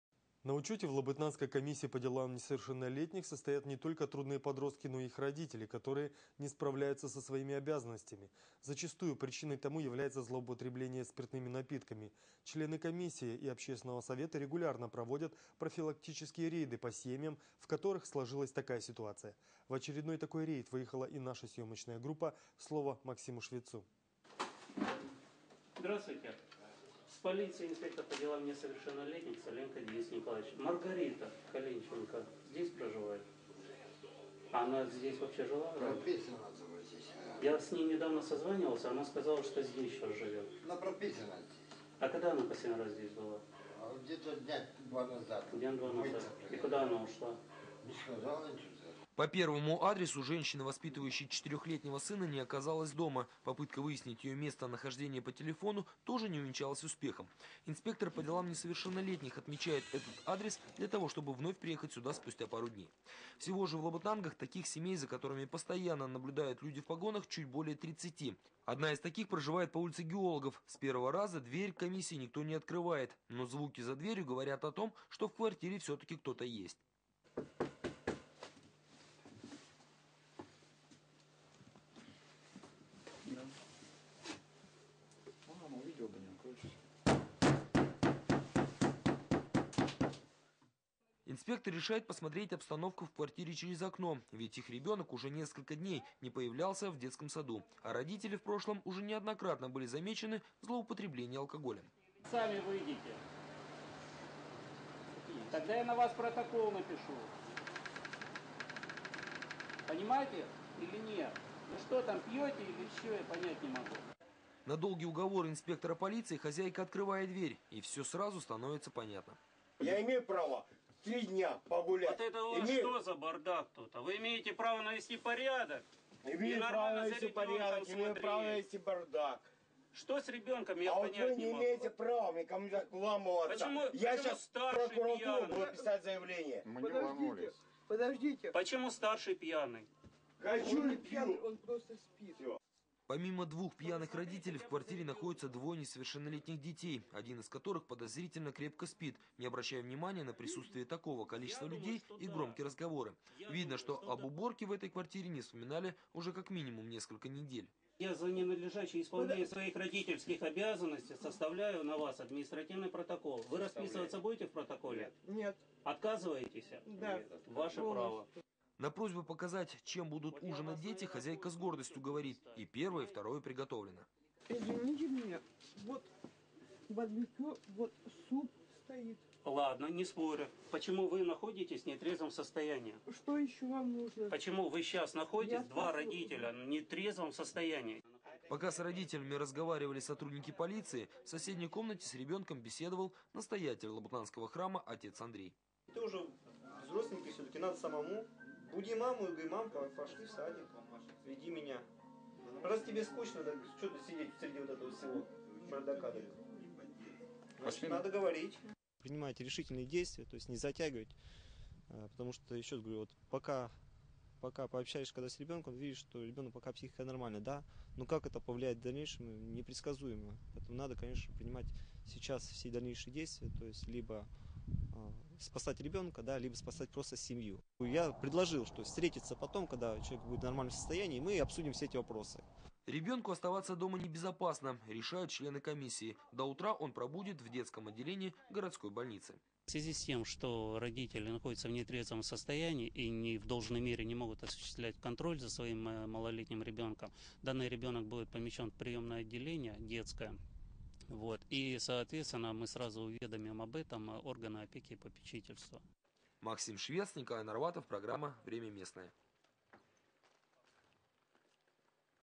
Родителей не выбирают
/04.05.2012/ На учете в лабытнангской комиссии по делам не совершенно летних состоят не только трудные подростки, но и их родители, которые не справляются со своими обязанностями. Зачастую причиной тому является злоупотребление спиртными напитками.